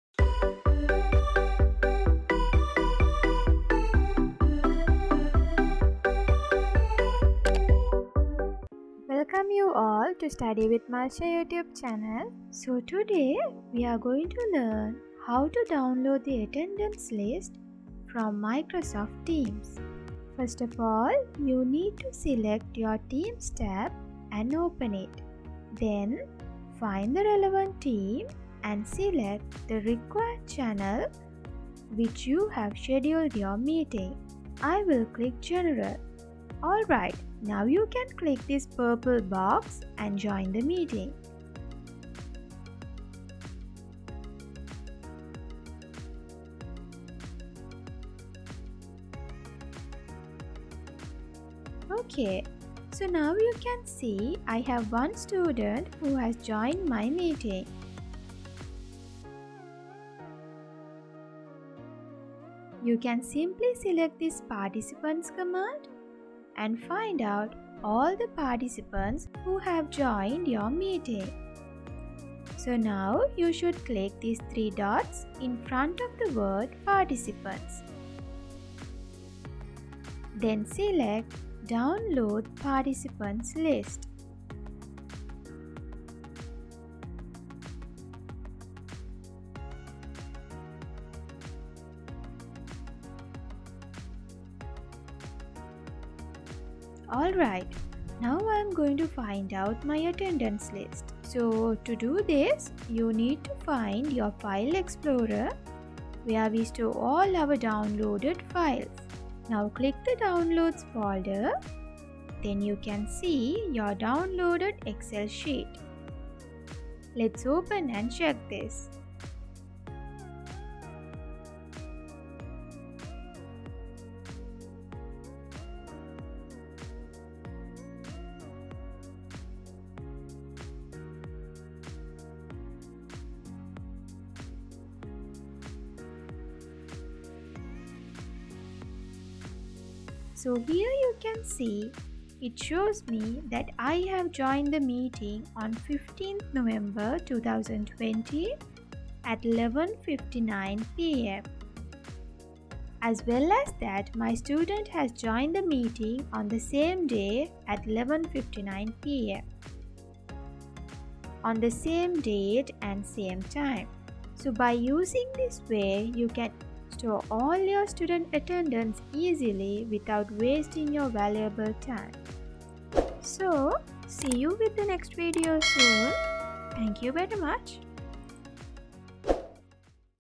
How to download the attendance list with MS Teams
So here with this video, you can learn how to download the attendance list with MST

***Specially dedicated for teachers and all organizers***
Try and share your ideas with us!

You can get easy and quick tips when using MS Teams with the below video links.